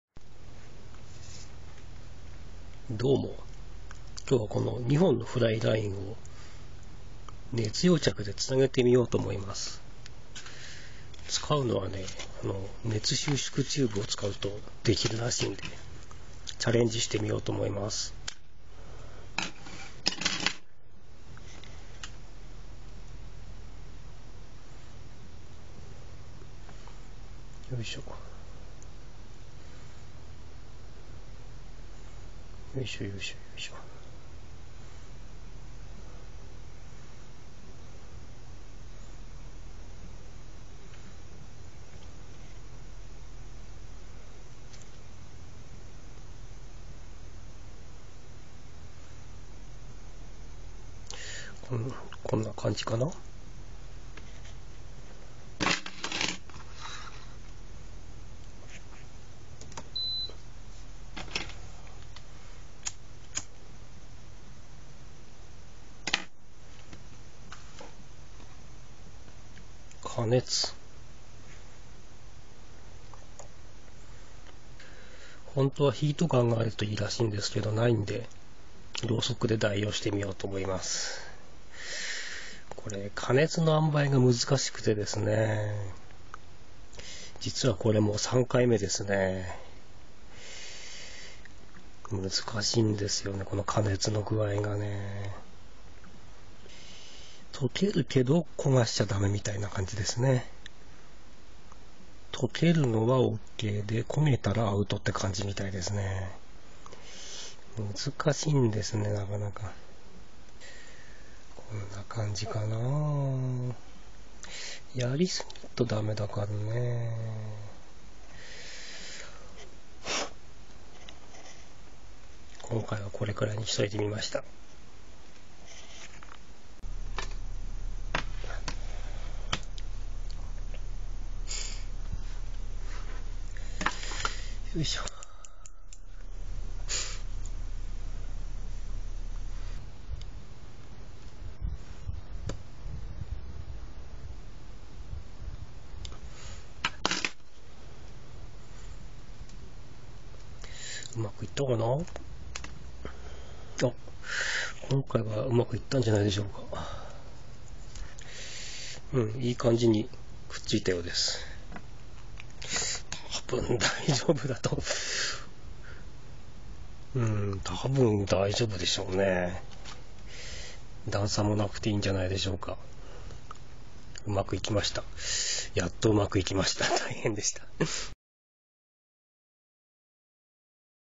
フライラインの熱溶着にチャレンジしてみた
Timecoフライトライ付属の専用ラインに使い古しのWFランニング部を接合してみました。
It is heat welding about a fly line and a fly line.